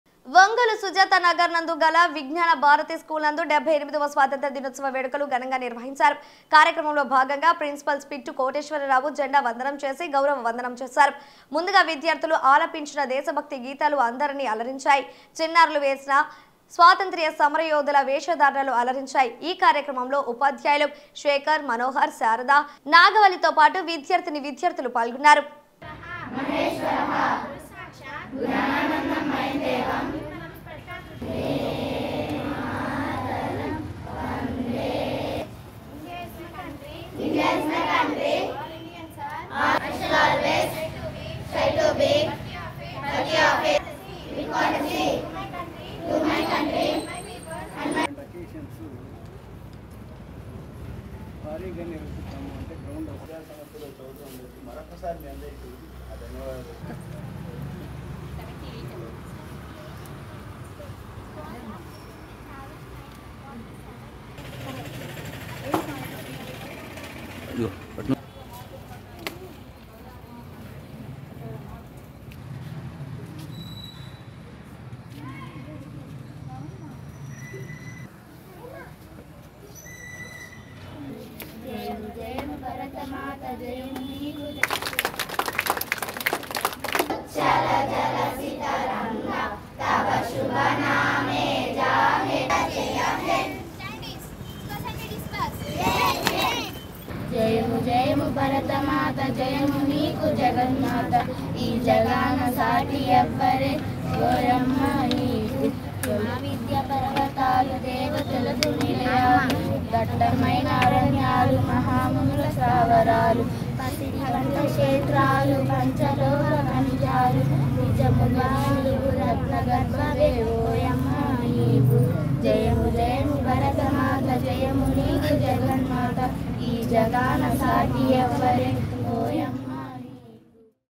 విజ్ఞానభారతి స్కూలు నందు ఘనంగా 78వ స్వాతంత్ర దినోత్సవ వేడుకలు
Ongole | Siti Cable | SITI ONGOLE NEWS